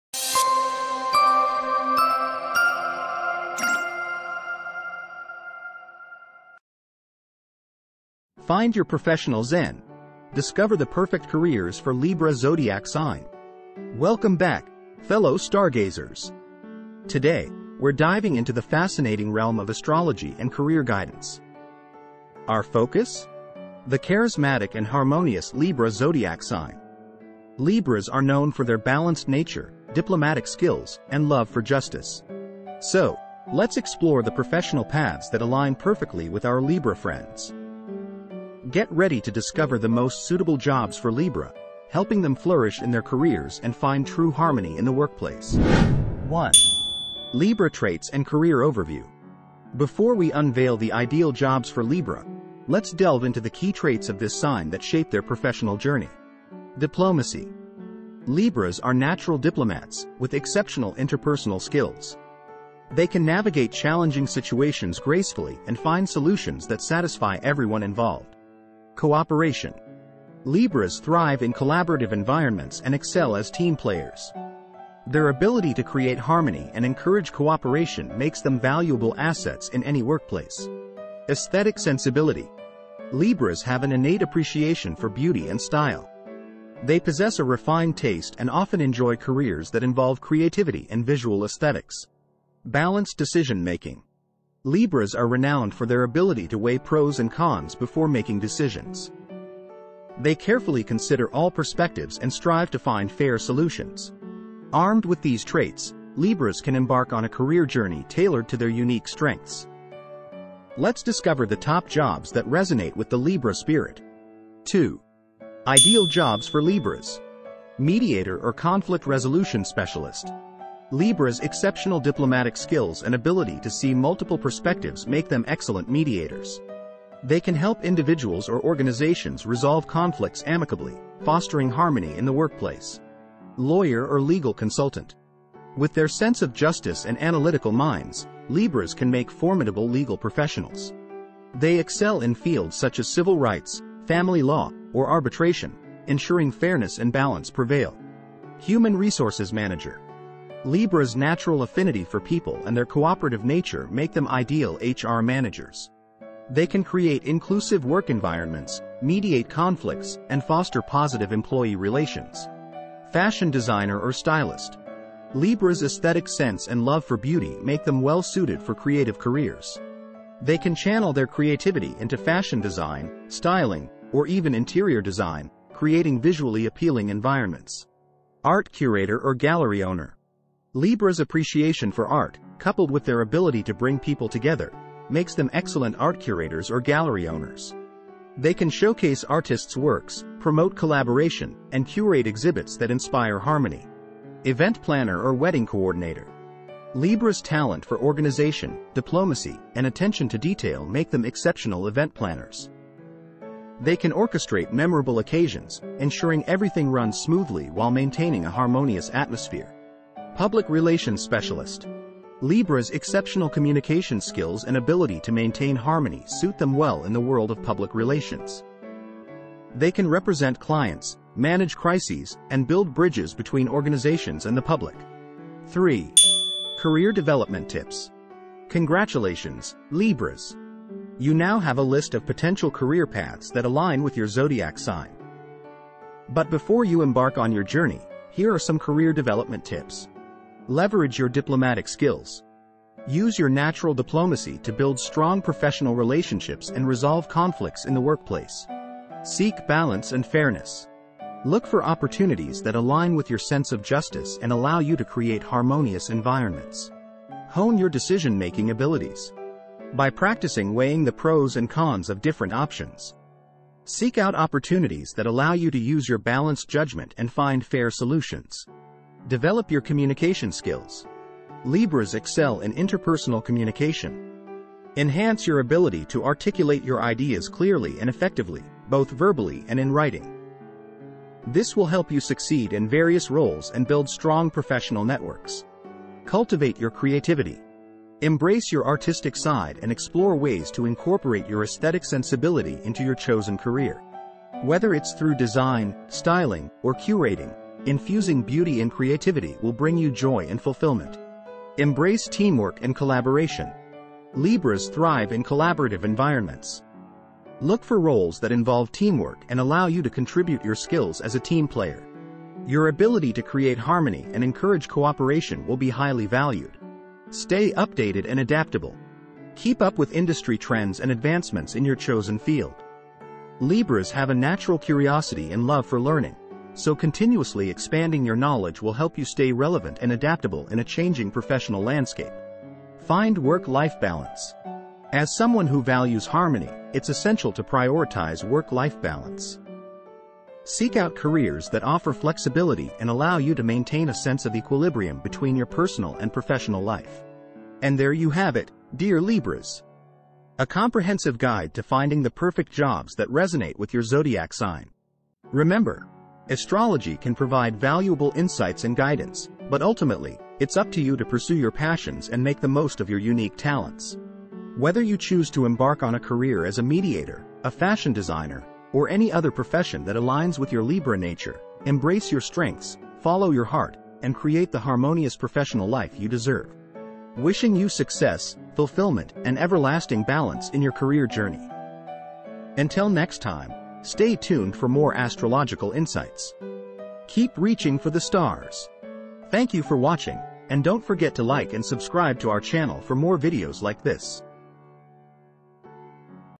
Find Your Professional Zen: Discover the Perfect Careers for Libra Zodiac Sign!
Unlock your professional potential with our insightful video: 'Find Your Professional Zen: Discover the Perfect Careers for Libra Zodiac Sign!' Dive into the world of astrology as we explore the traits of Libra, known for their diplomacy, balance, and love for harmony. Gain valuable career guidance and uncover the ideal jobs that align with your Libra spirit. Whether you're seeking a career in mediation, fashion, law, or beyond, this video provides the roadmap to finding career fulfillment and creating a harmonious work-life balance. Don't miss out on this transformative journey of self-discovery and career success. Watch now and embark on your path to professional zen!